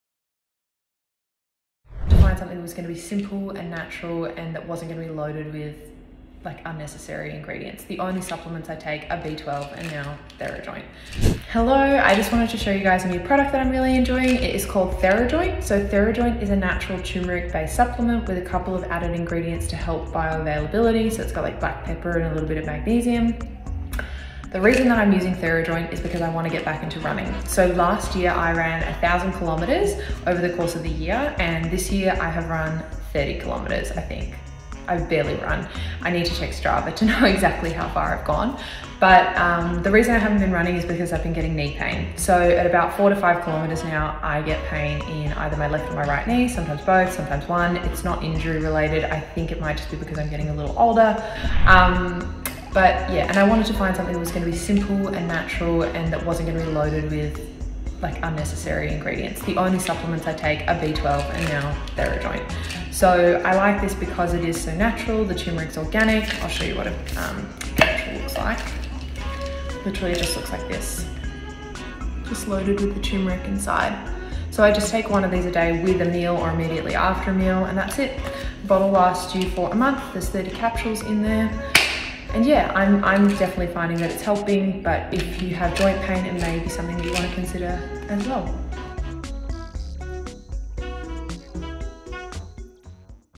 I just wanted to show you guys a new product that I'm really enjoying. It is called Therajoint
Therajoint is a natural turmeric bio- supplement with a couple of added ingredients to help bioavailability. So it's got black pepper and a little bit of magnesium